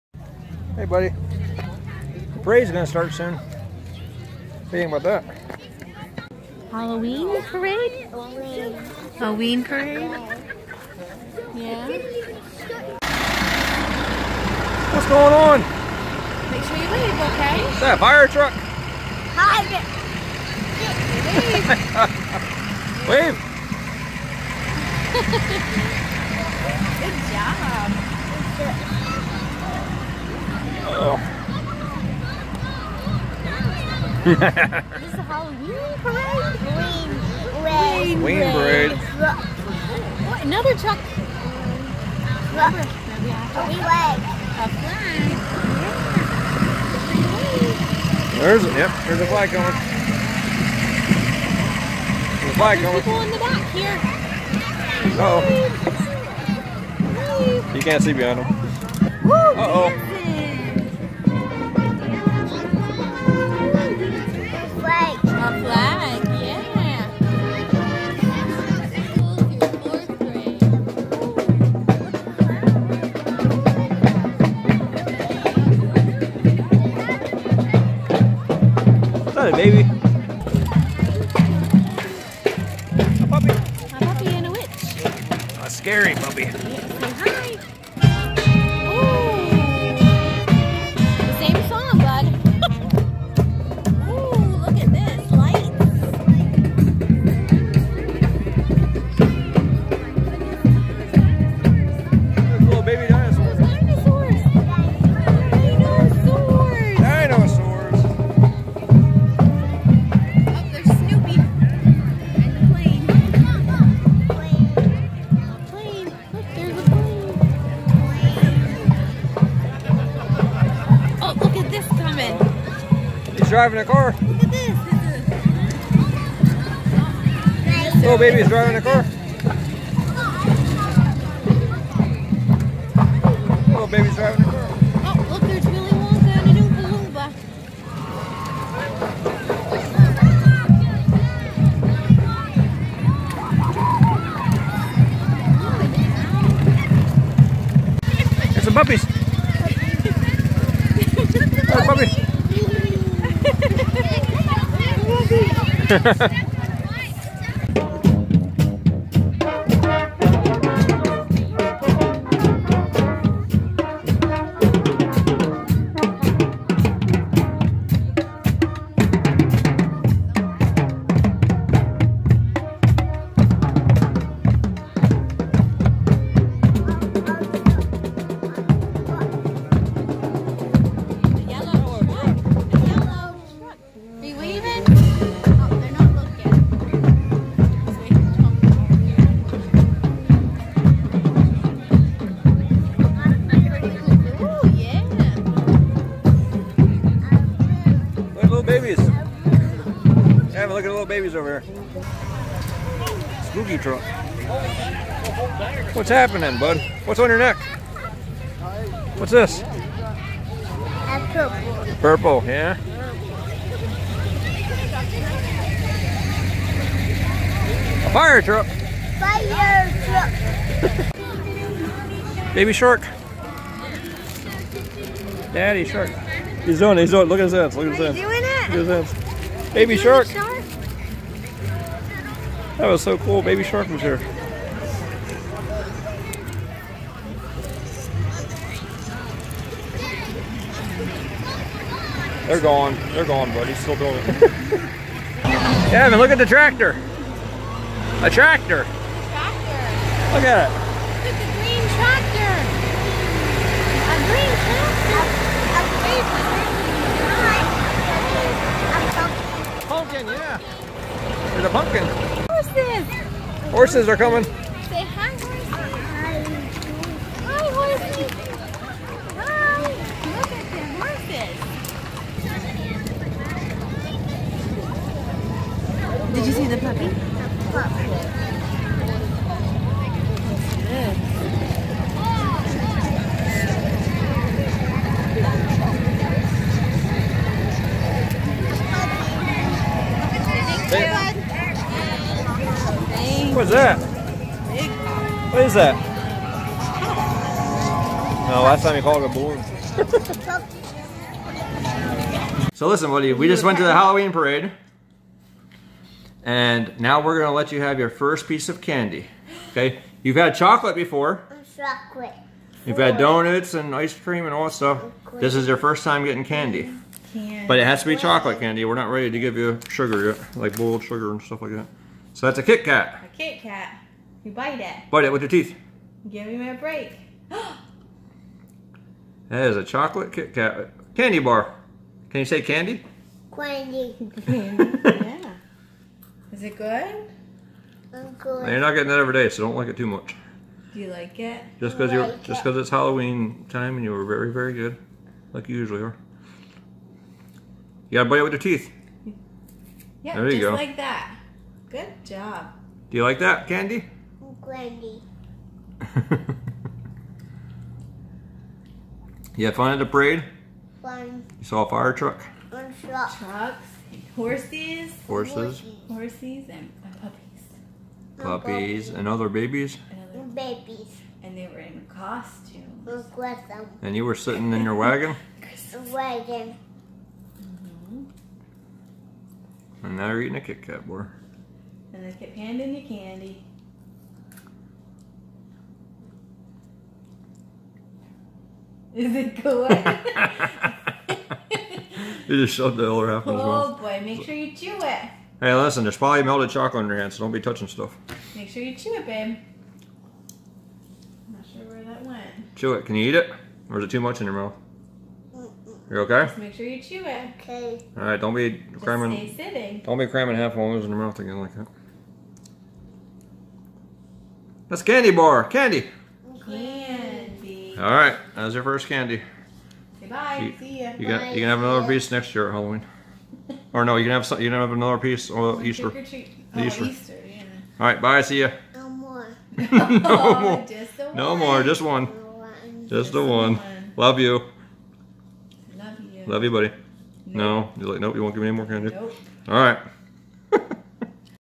Little Gavin Baby At The Halloween Parade And Eating First Candy Bar! (Recorded 10/22/2018)
Baby Gavin taking in the Halloween parade, and afterward officially eating his first candy bar!  He was 25 months old at the time.